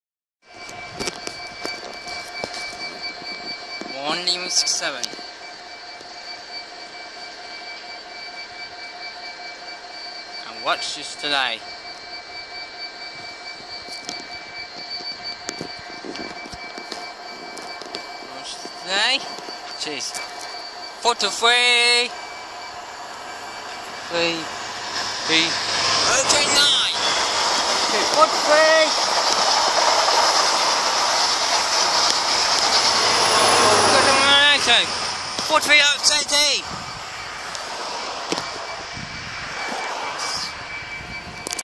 43020-43070 1L67 at Greenbridge Swindon Wiltshire 4.7.2017
Great Western Railway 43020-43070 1L67 bound for London Paddington PAD from Cheltenham Spa CNM to London Paddington PAD